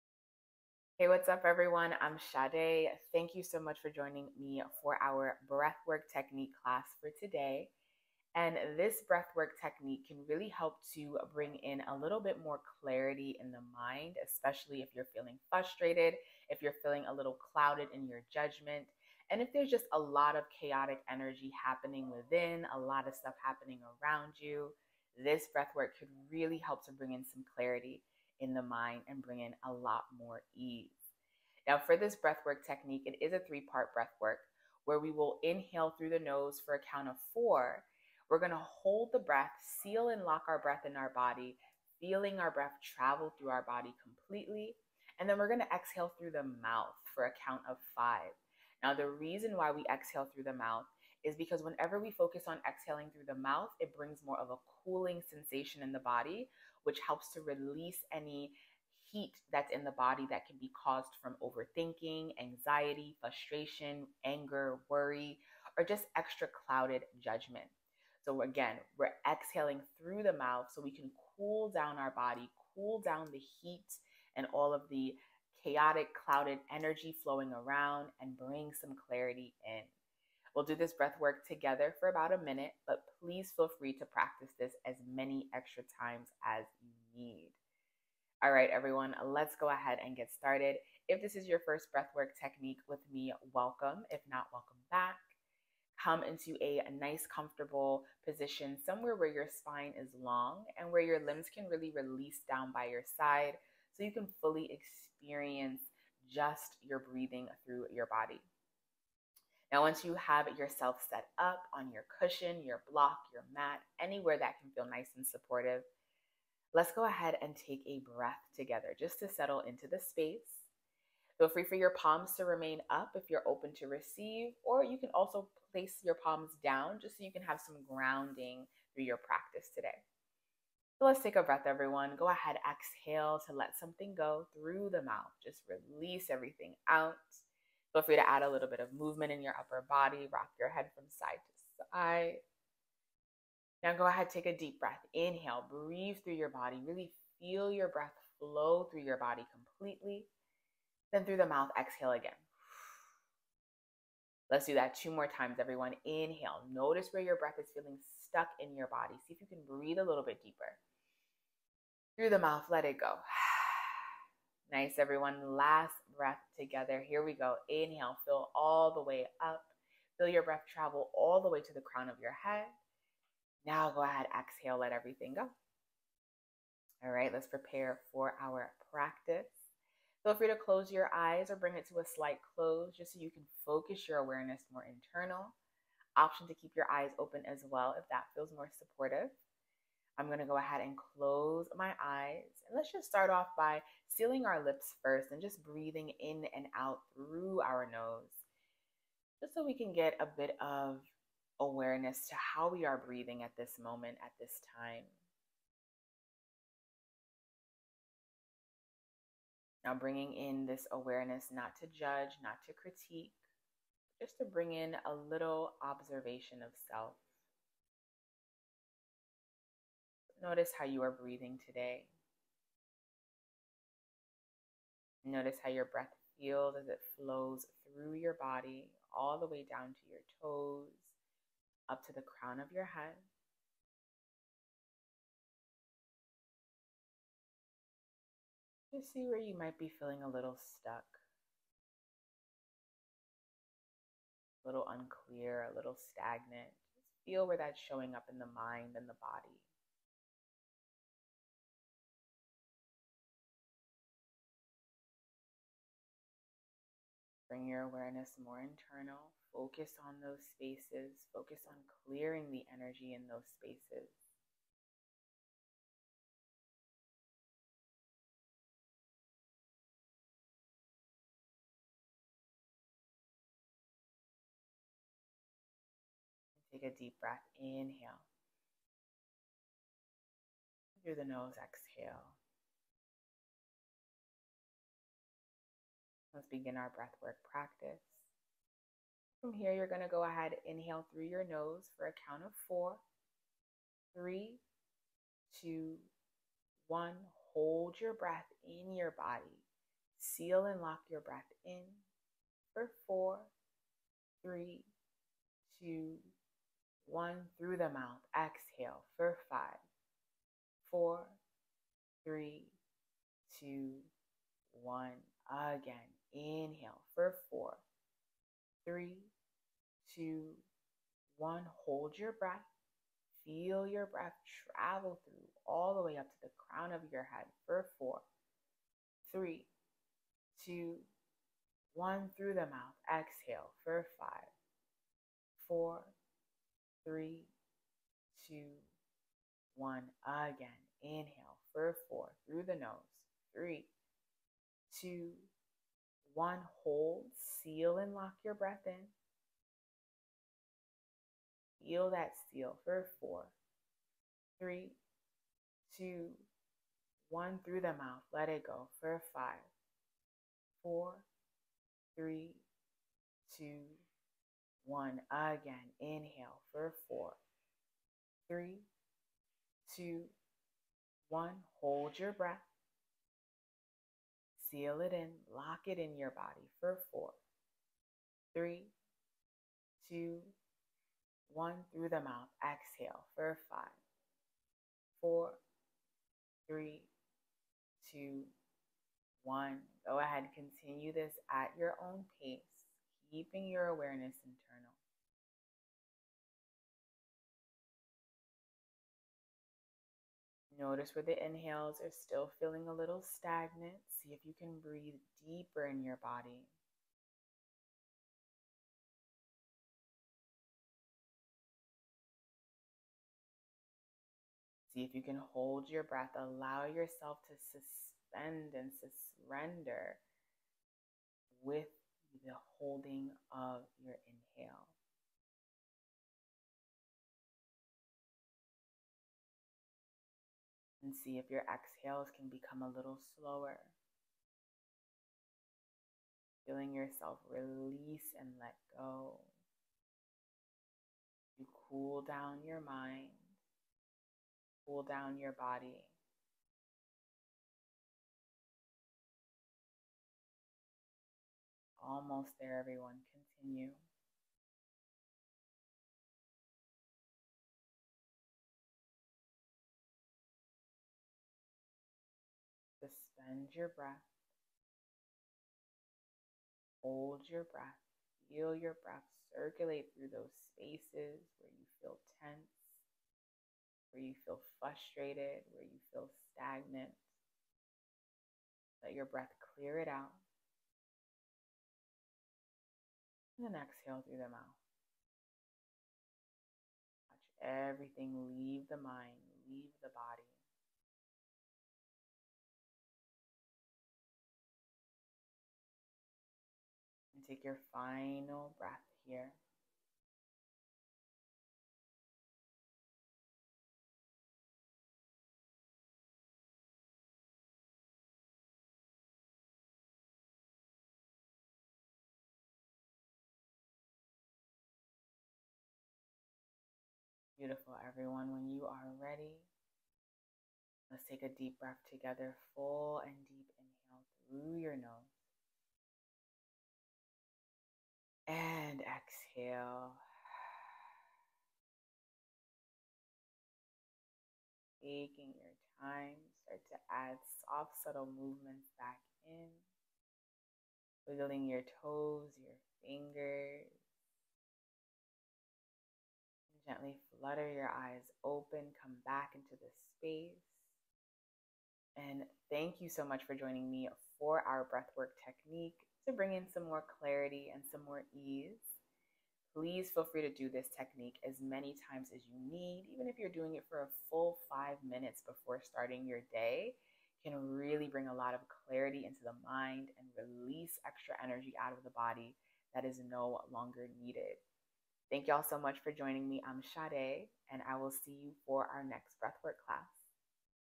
Cooling Breathwork for a Calm Mind
Join me for a 3 part Breathwork technique that brings a cooling sensation and effect on the mind!